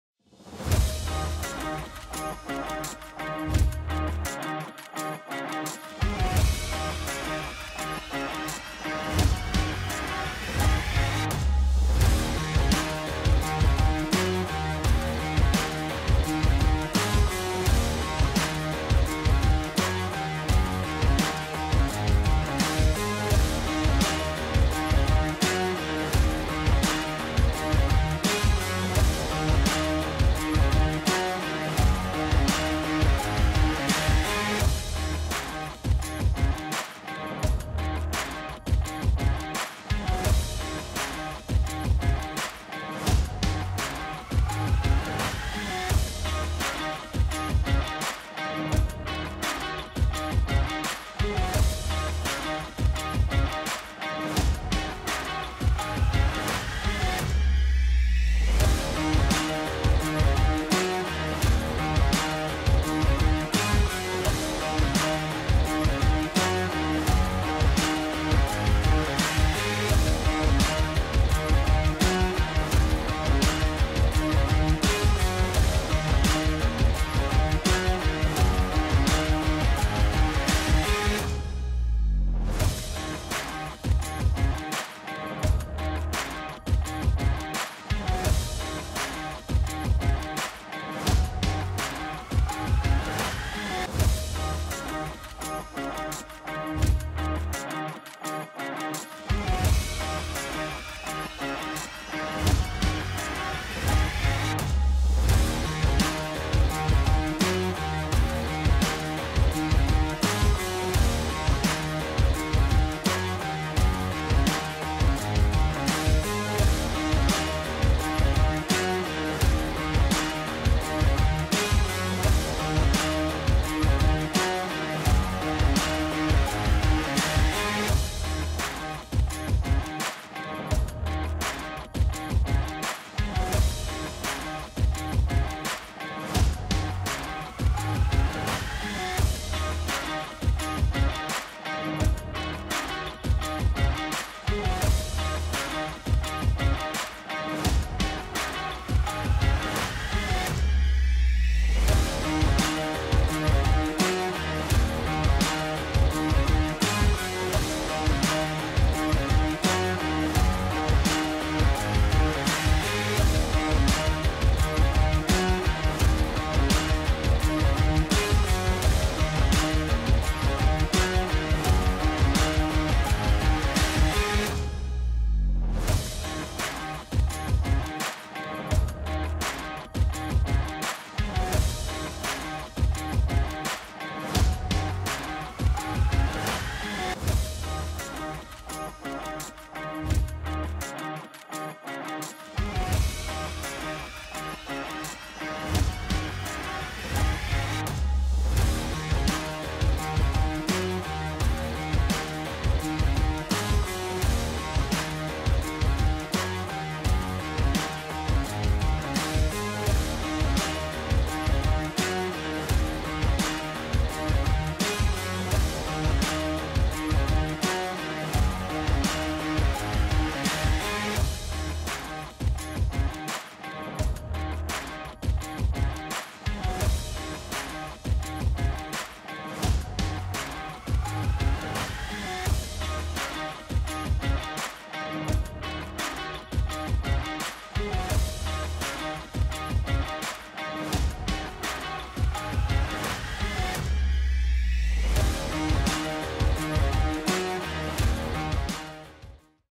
TRYLIGHTS | Leicester Tigers vs Coventry Rugby | 09.11.2024 | Premiership Rugby Cup
Trylights of our Premiership Rugby Cup Round 2 fixture against Leicester Tigers at Mattioli Woods Welford Road!